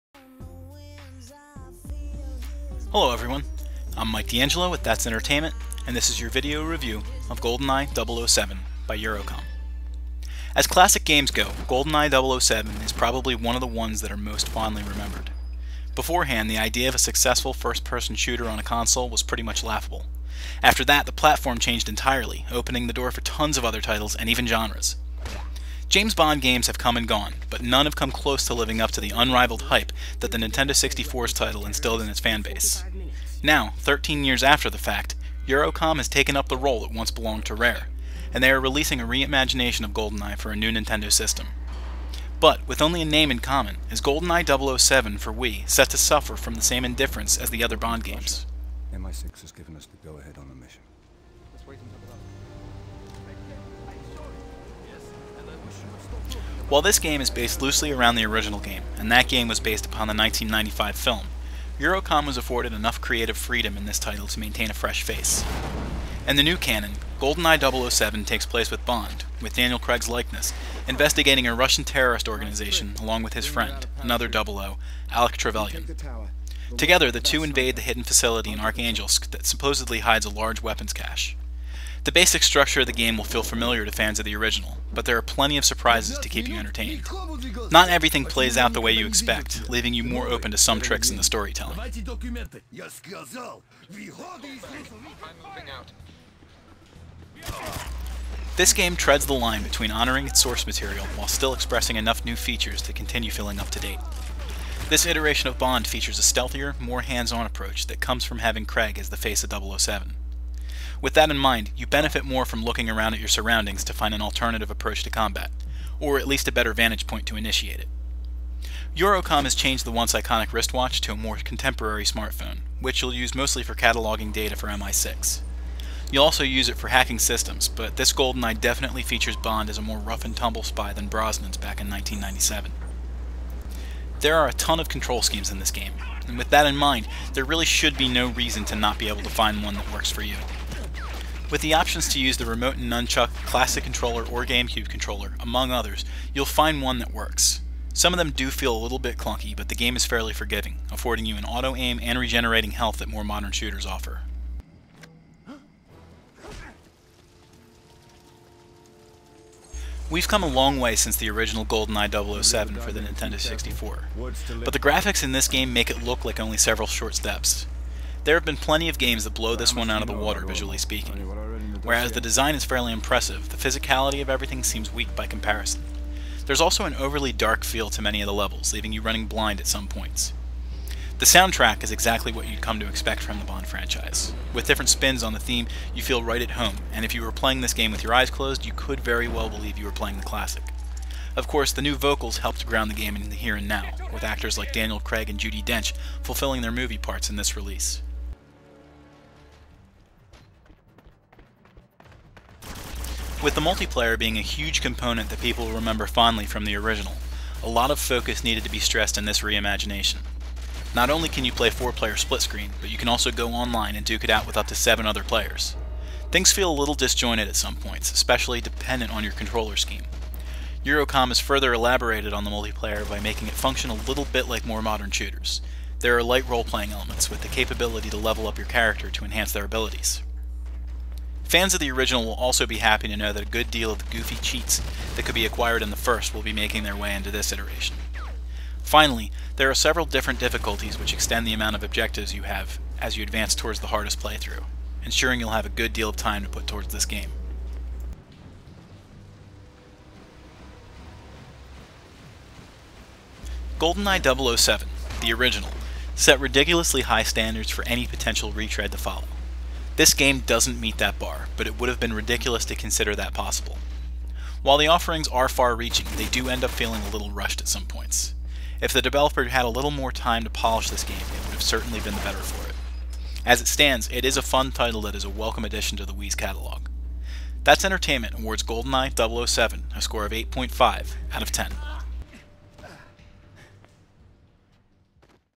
GoldenEye 007 Review - That's Entertainment
Mike DeAngelo, of That's Entertainment's review of GoldenEye 007, by Eurocom. 8.5/10